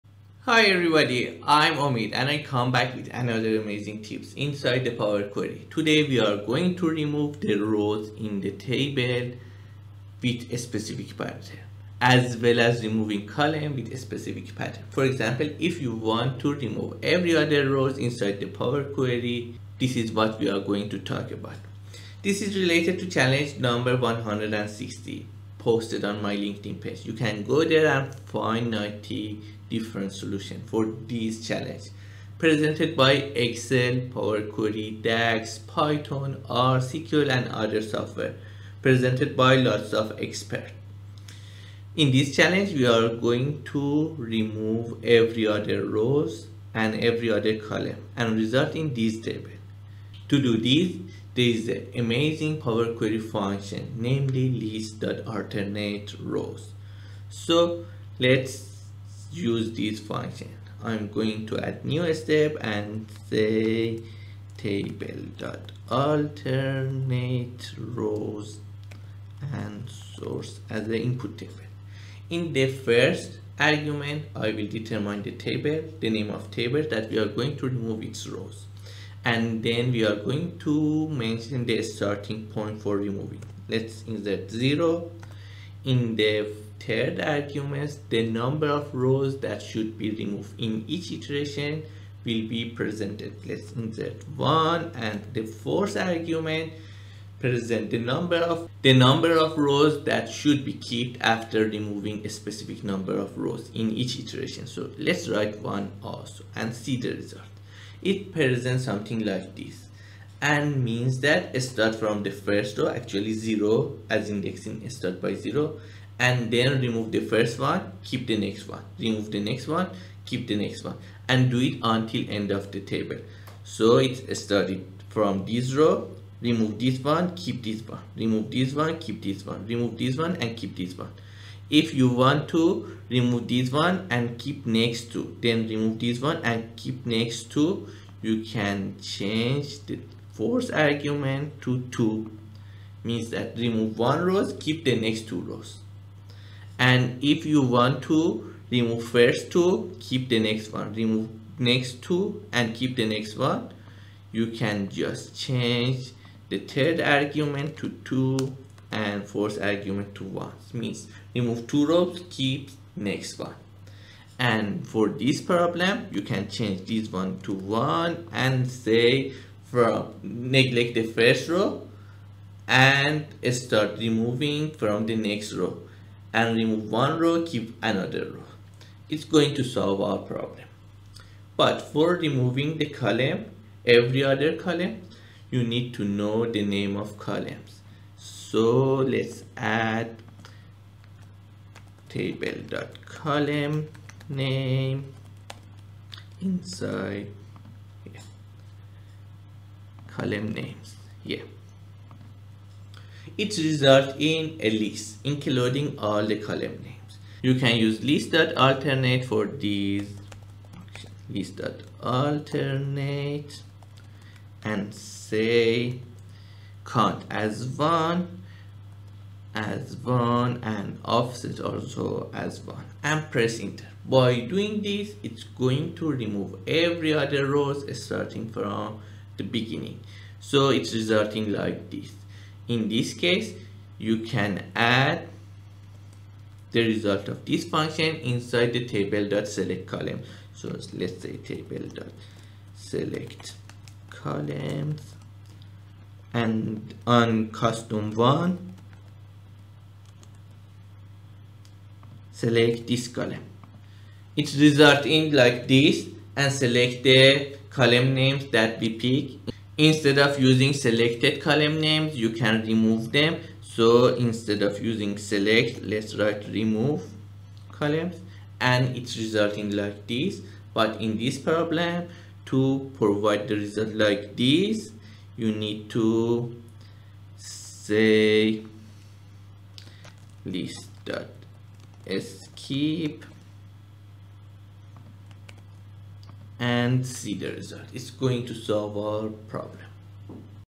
Remove Rows & Columns With Less Known Power Query Functions, Table.AlternateRows - Challenge 116
In this video, we solve Challenge 116: Removing rows and columns in Power Query using the less-known Table.AlternateRows function. Learn how to ignore the first row and column, and then remove every other row and column in the dataset. This tutorial is ideal for mastering efficient table manipulations in Power Query!

00:00 Introduction
01:02 Power Query Editor
02:40 Fourth argument of function